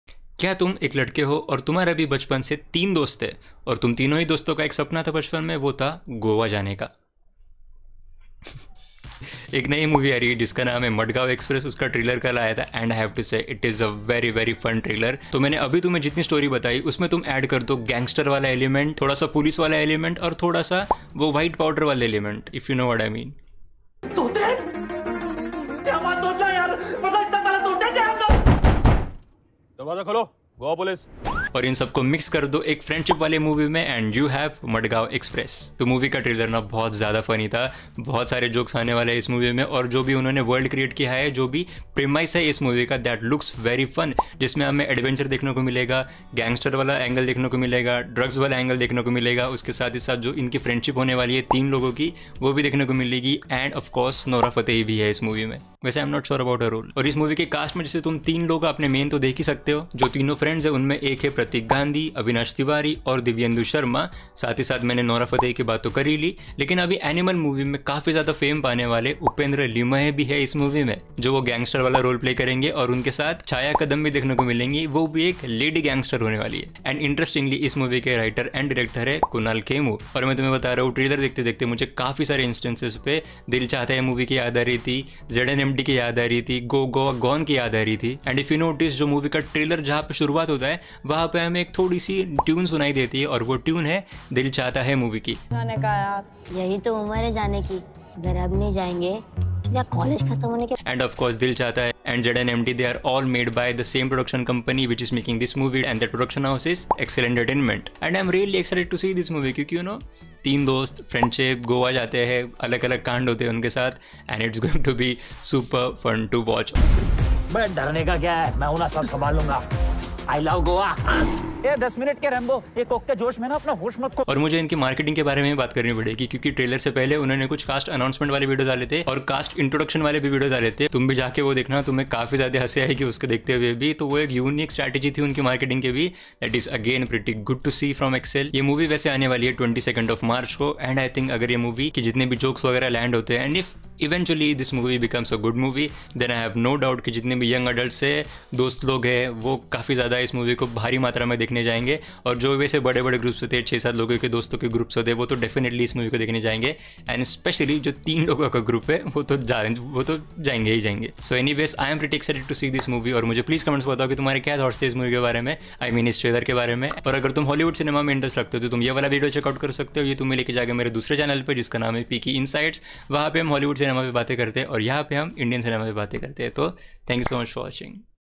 Madgaon Express Trailer Review | Peaky Insights Hindi
Madgaon Express is a upcoming movie and here is its Trailer Review

English Channel (Hollywood Cinema):
Peaky Insights: